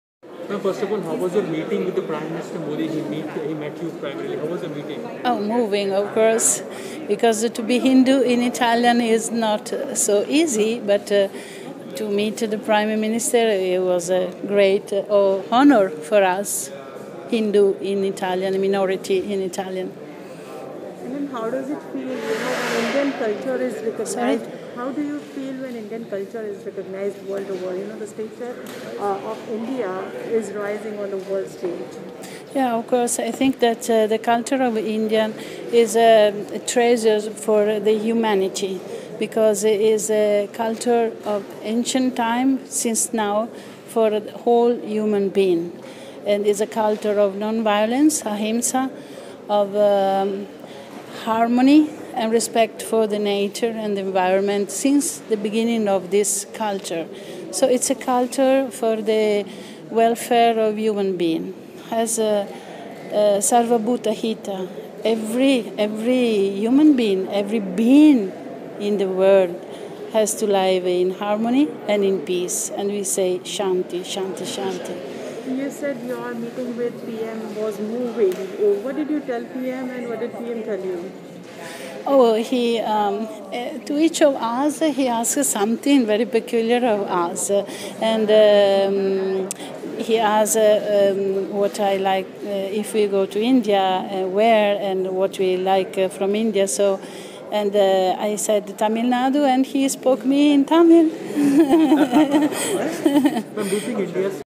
Svamini Hamsananda Giri shares her experience of interacting with PM Modi in Italy | PMO
Svamini Hamsananda Giri shares her experience of interacting with Prime Minister Narendra Modi in Italy